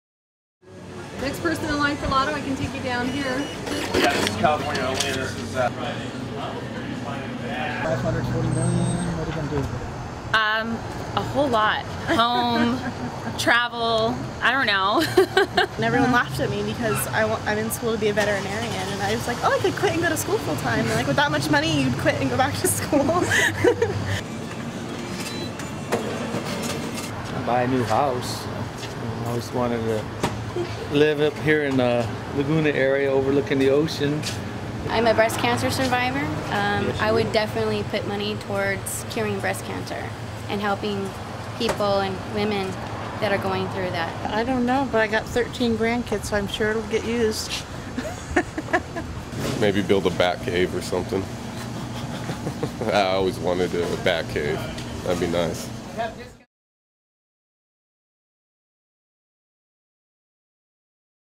What would you do with the Mega Millions jackpot? - 2012-03-29
We asked lottery players outside the Baycrest Liquor store in Costa Mesa what they would do with the $540M+ Mega Millions jackpot. Video by Eugene Garcia, ocregister.com Published on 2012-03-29